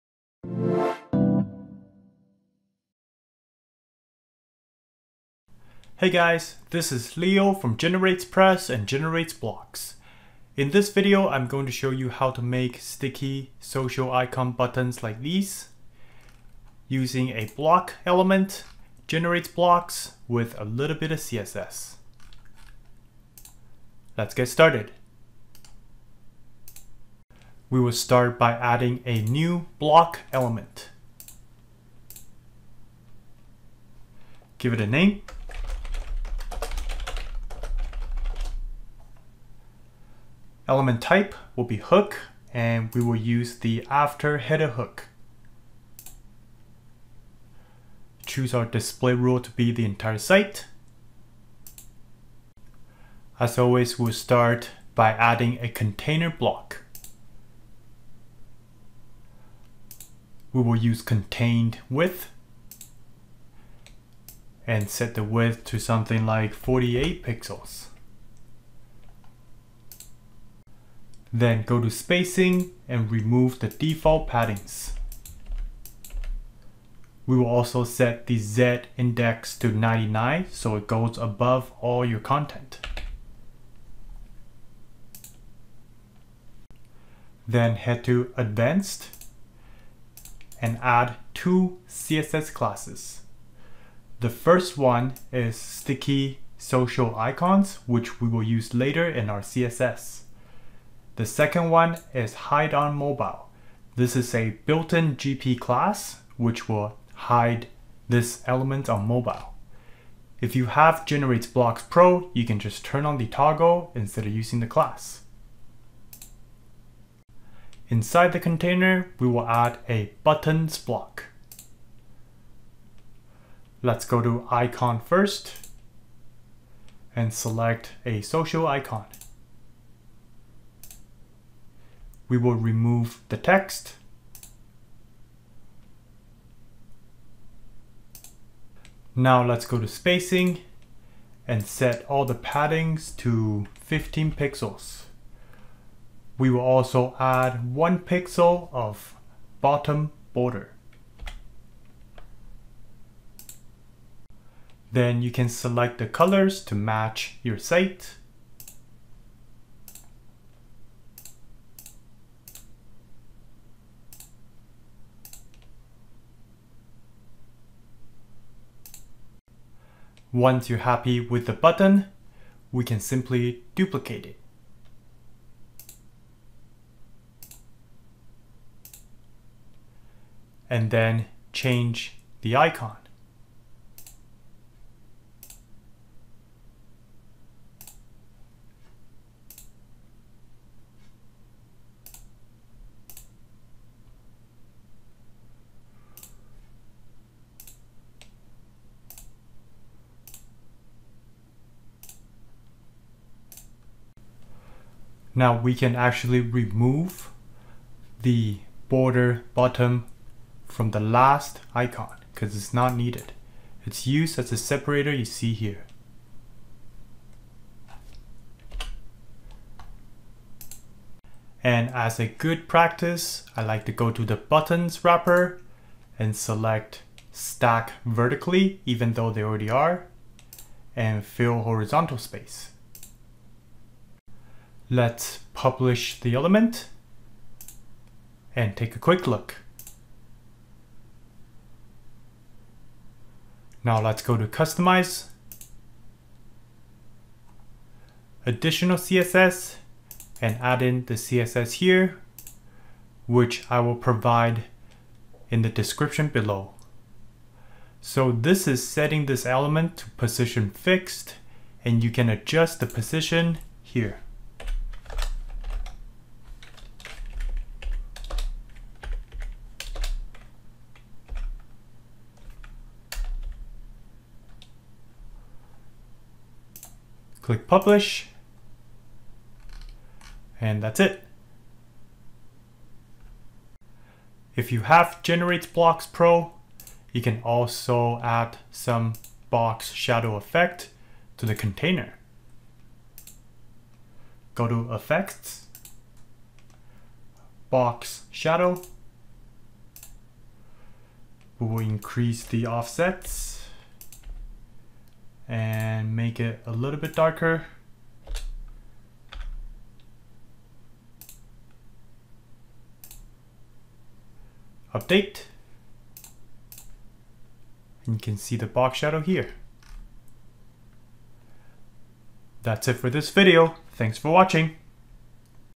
GeneratePress - Sticky Social Icons with Block Element
This video demonstrates how to create sticky social icons with a block element and GenerateBlocks.